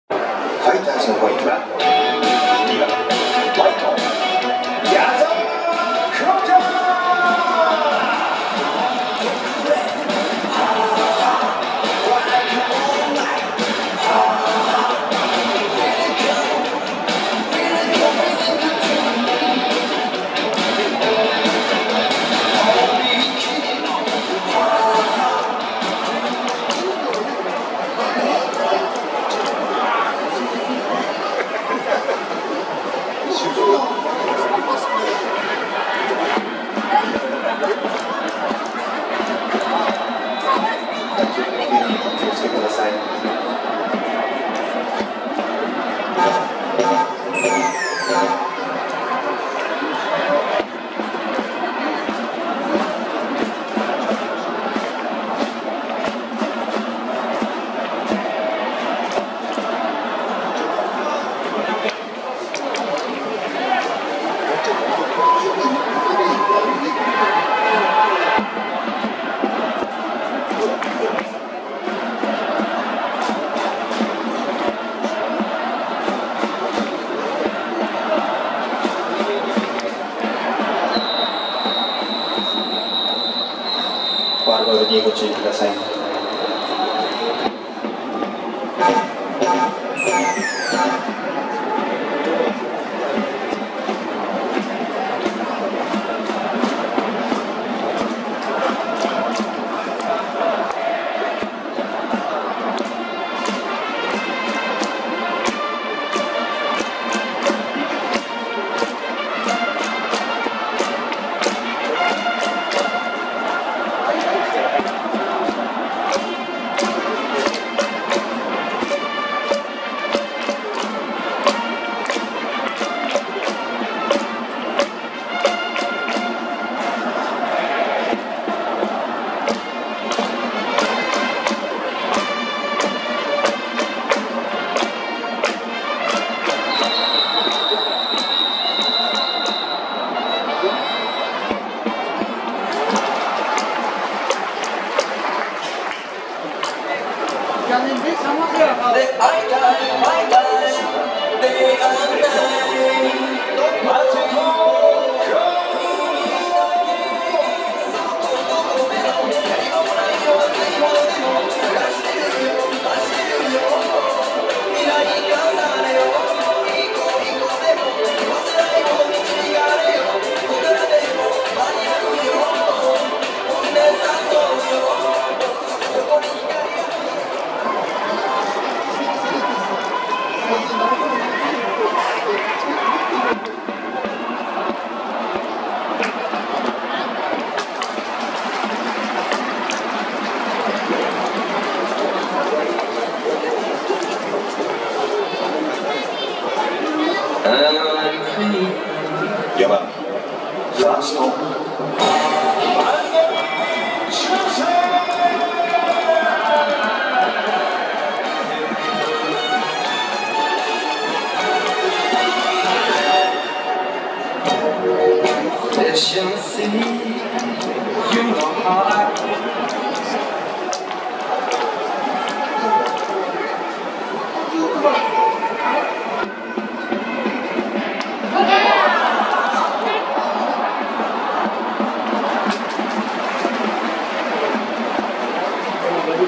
20230523【田中正義】7回裏 味方の逆転を信じて準備を進めるジャスティス様
超望遠で無理矢理撮ってます。
エスコンフィールド北海道のソフトバンク戦